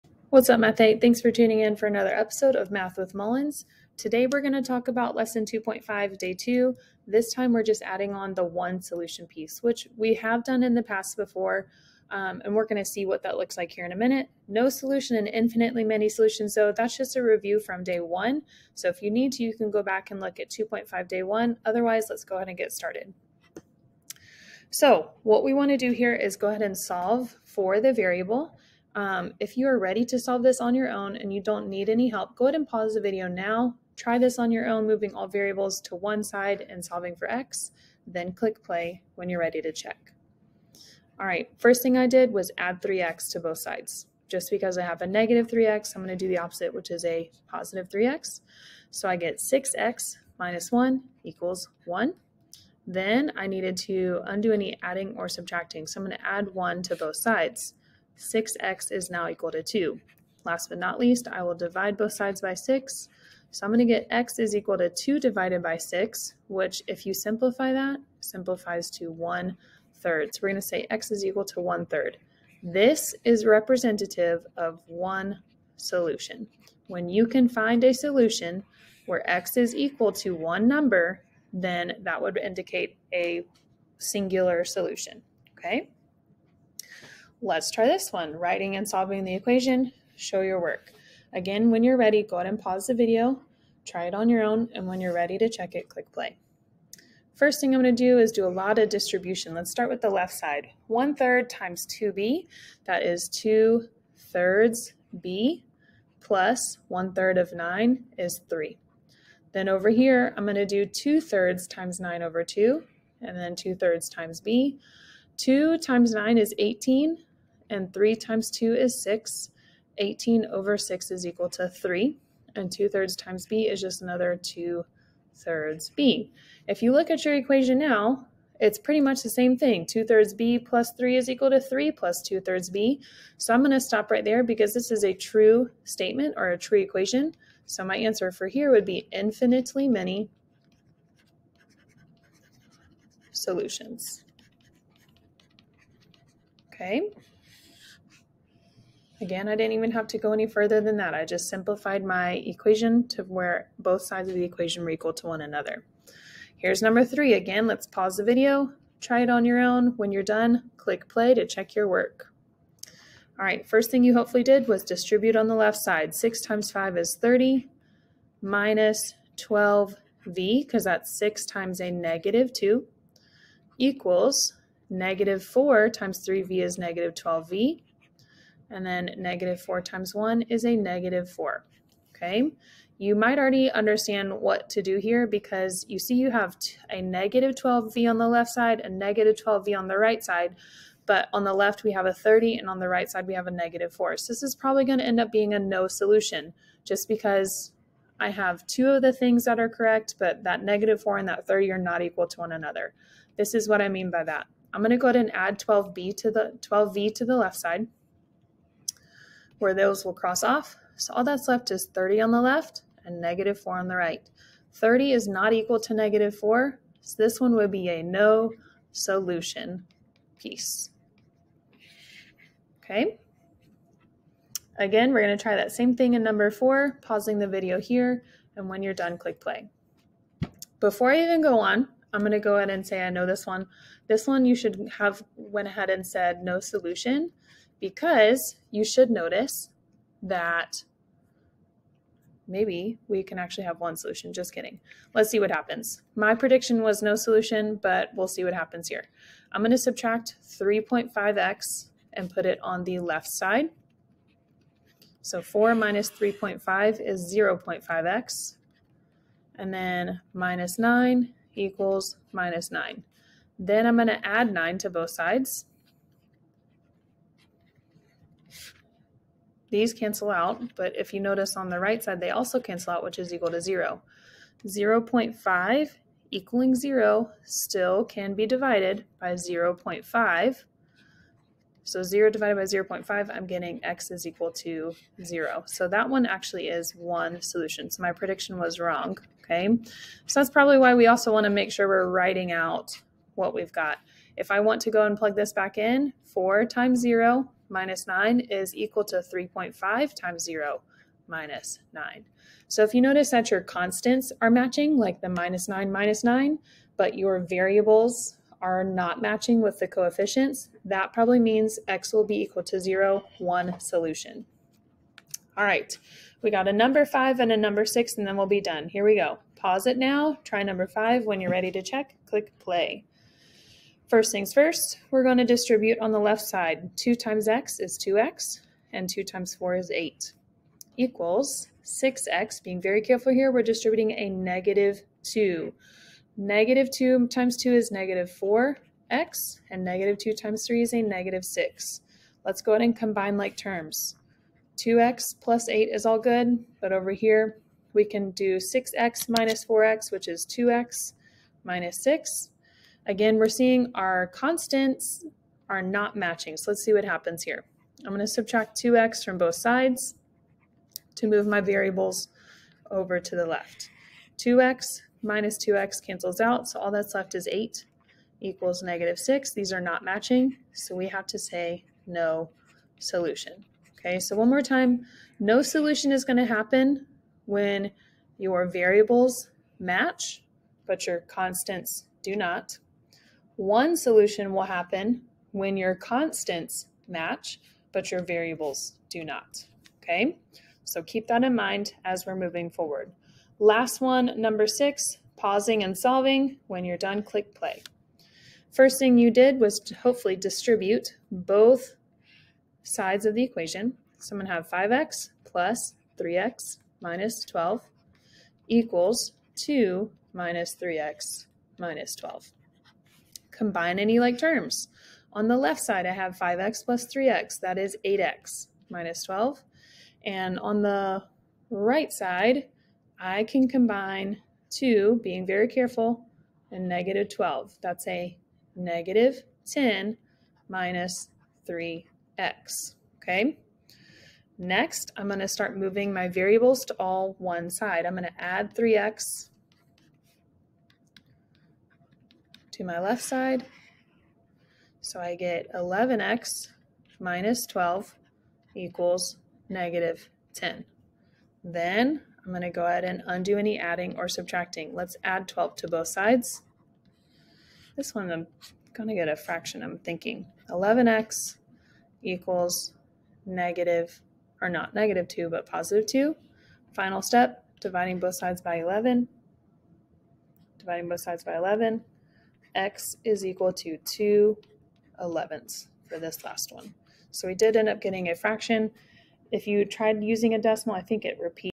2.5 Infinitely Many and No Solutions (day 2)
More practice with Infinitely Many solutions and no solutions, but now let's see what happens when we see one solution!